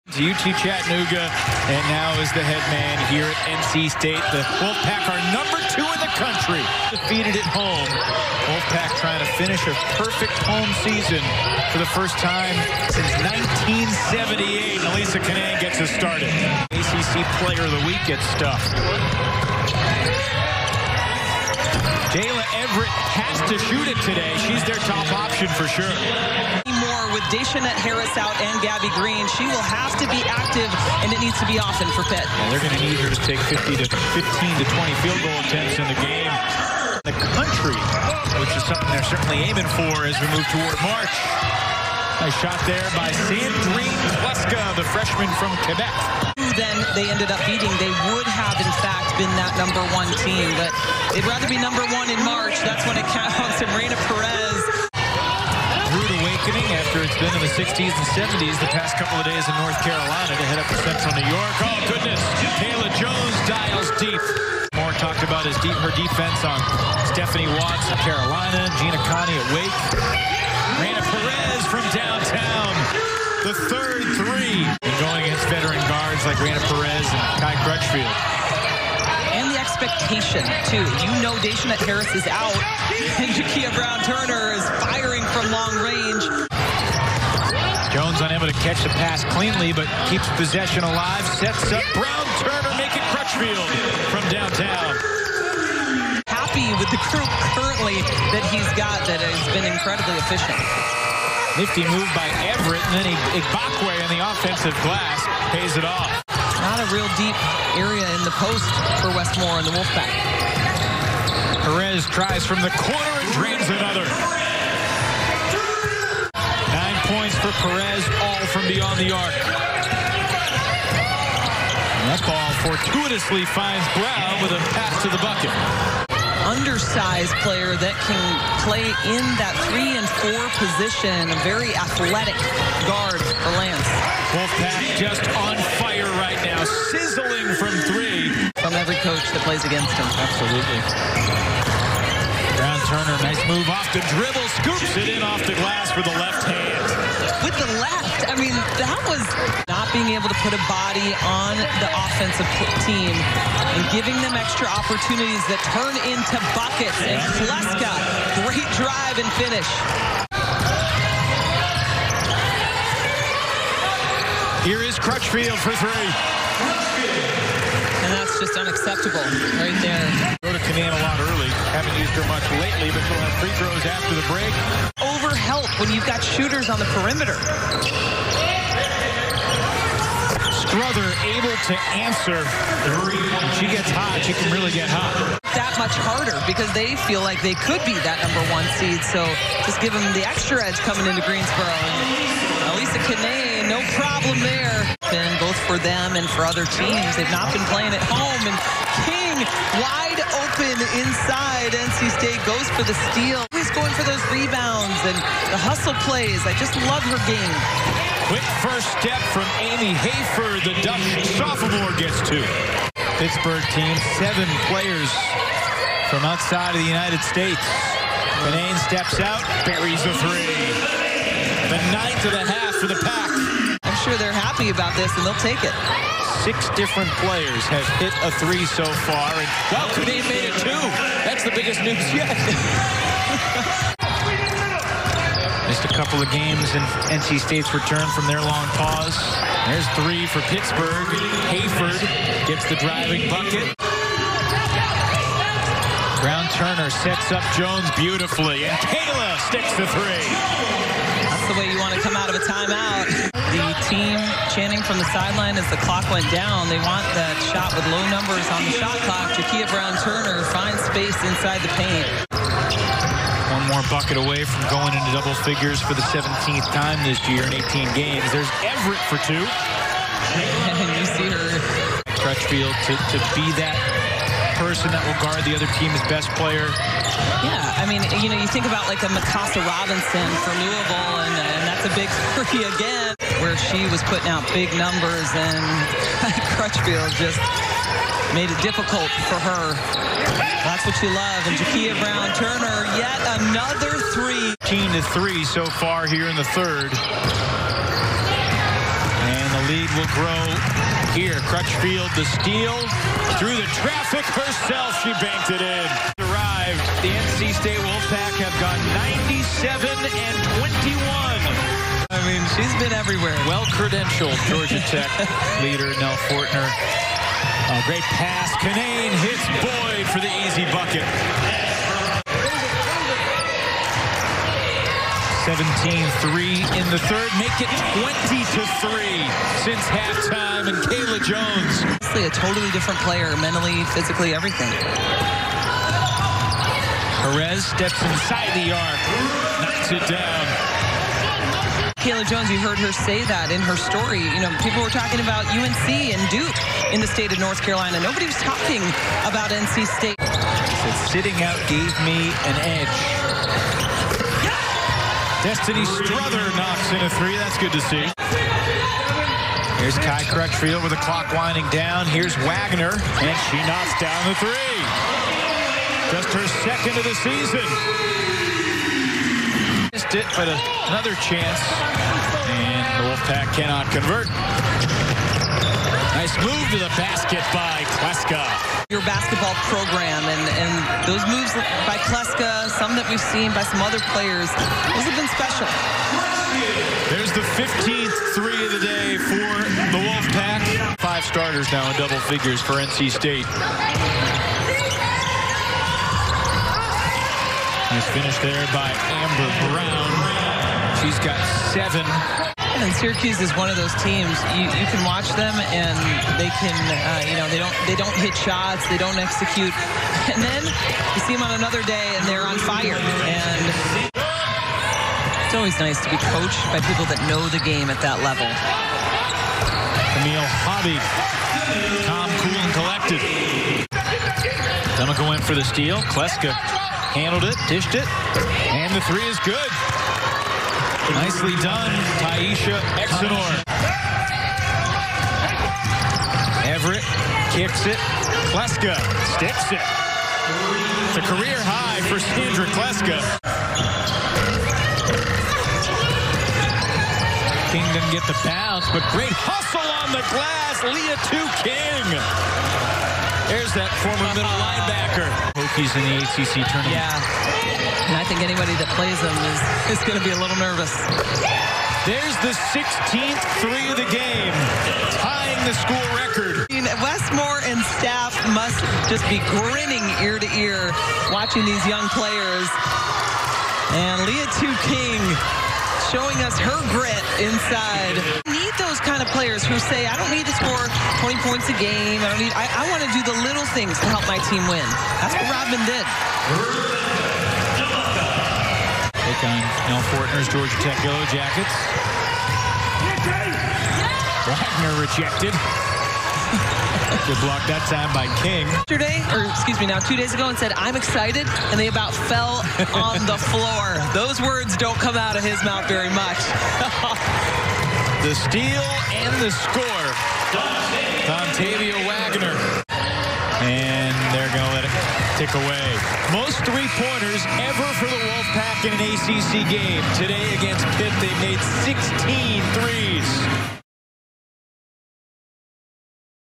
Pitt vs. NC State Condensed Game | 2020-21 ACC Women's Basketball
The No. 2 NC State Wolfpack Women's Basketball team defeated Pitt, 83-53 in ACC play.  Jackia Brown-Turner led the Wolfpack in scoring with 14, Elissa Cunane grabbed 8 rebounds and Kayla Jones dished out 7 assists in the win.  Sandrine Clesca led the Panthers with 12 points in the loss.